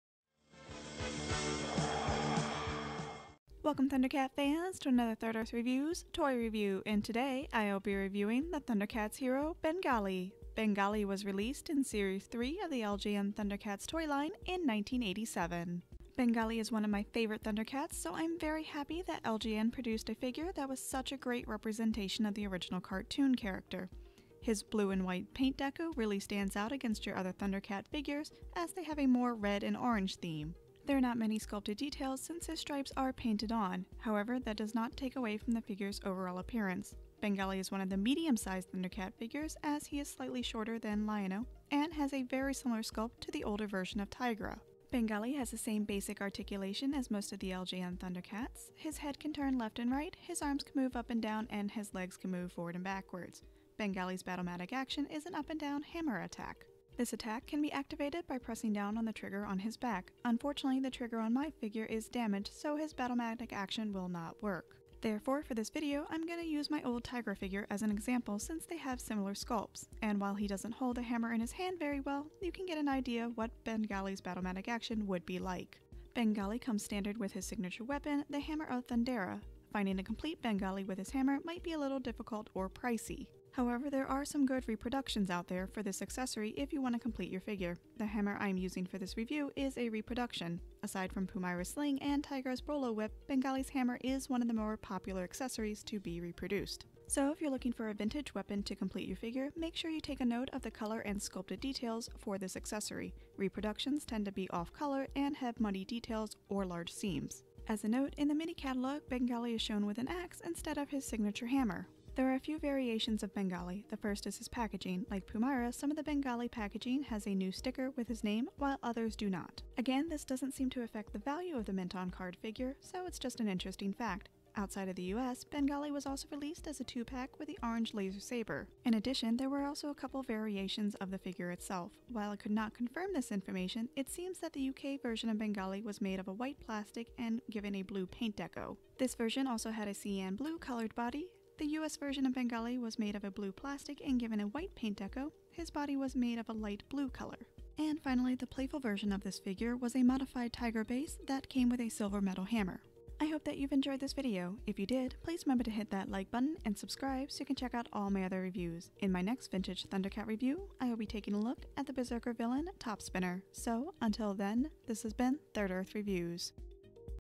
LJN Thundercats Toy Review: Bengali
A detailed look at the 1980s Thundercats hero Bengali from LJN. Includes packaging, accessories and variations.